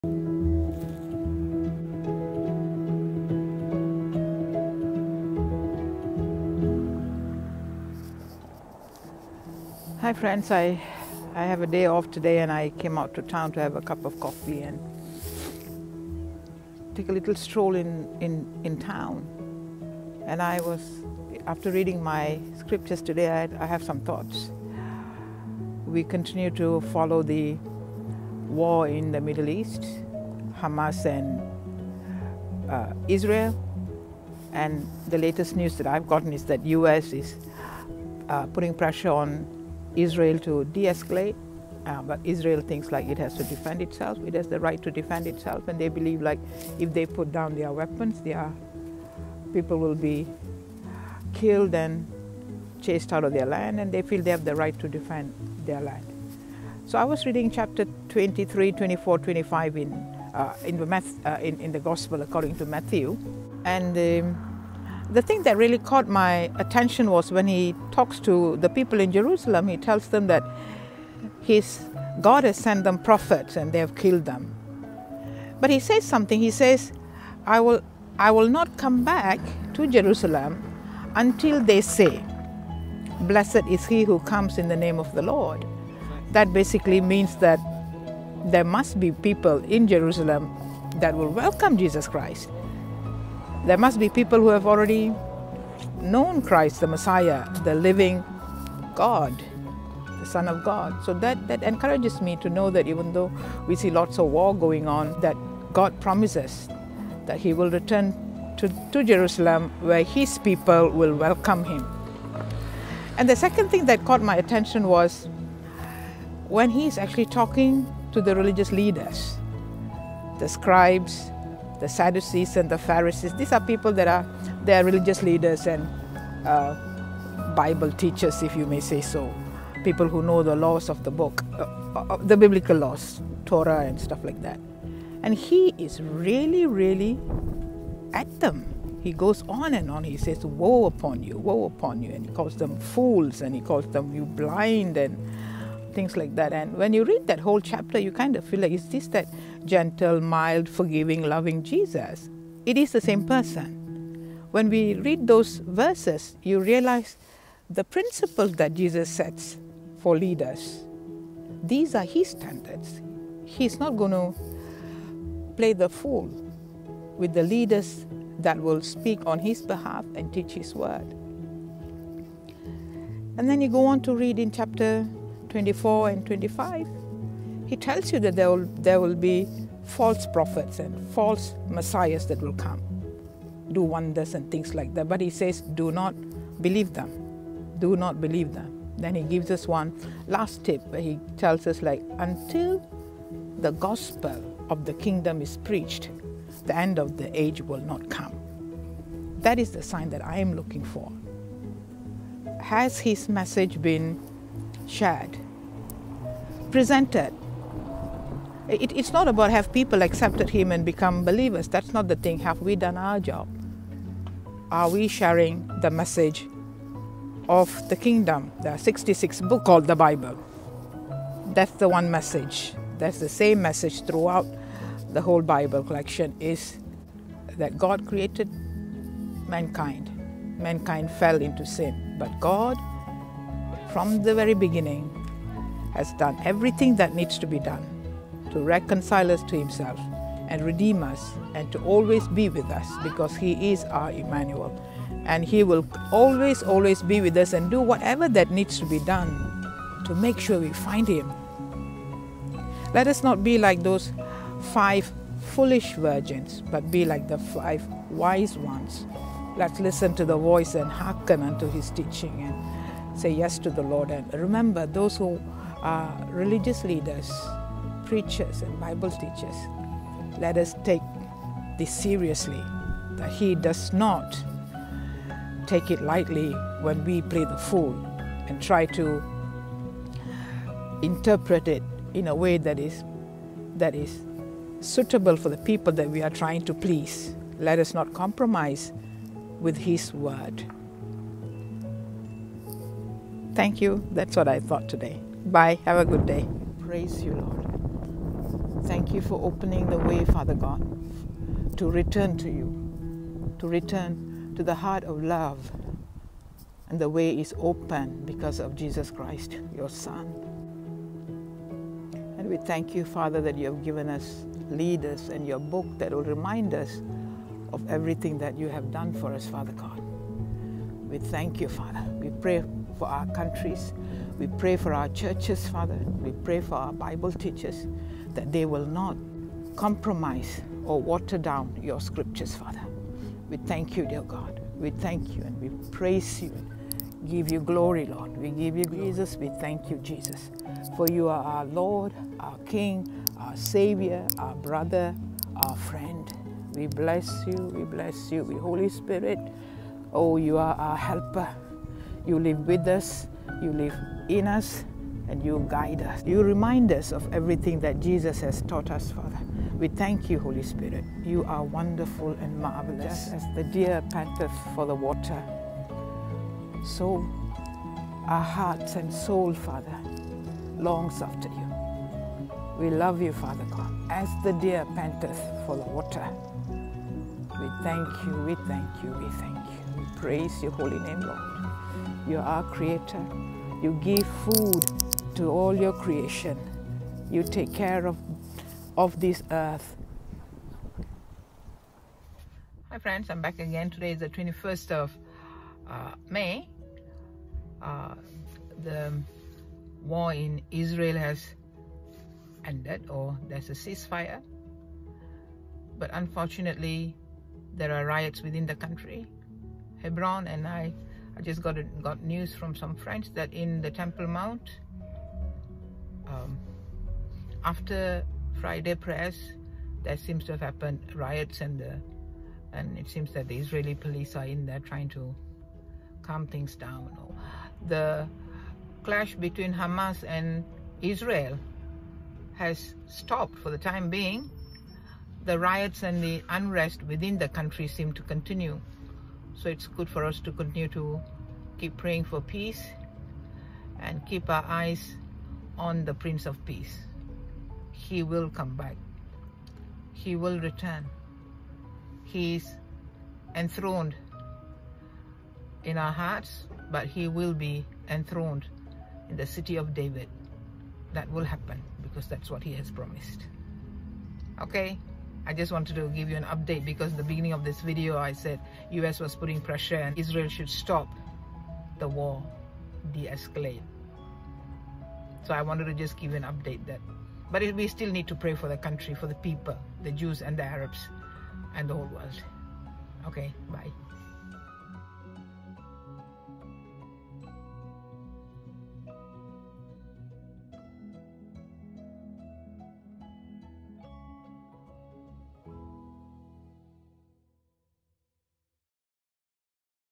Jesus warns in His Words.
End times and our attitudes as we wait for His return. Looking for signs, trying to predict , speculate. Let us not neglect His commands and warnings.